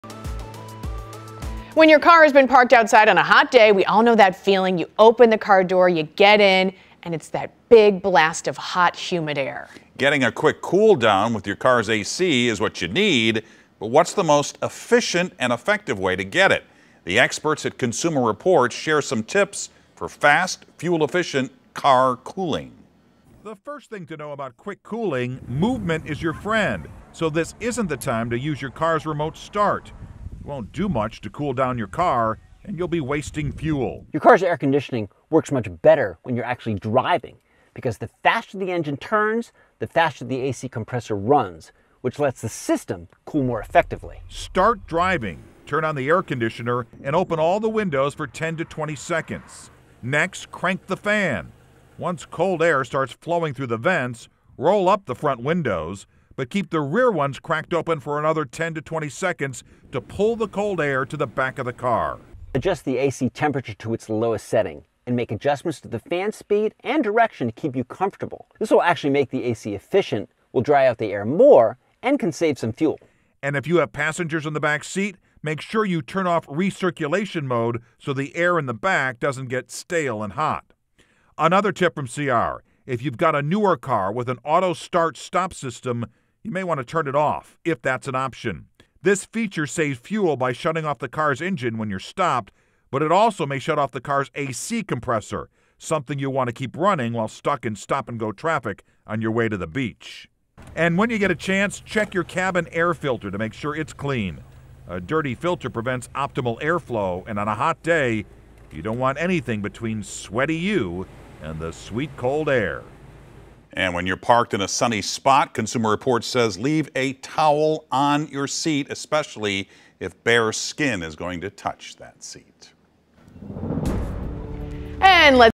Consumer Reports: The best way to cool down your car
There are a few ways to cool down your car that you might not think about. From windows to AC, here are a few tips from Consumer Reports.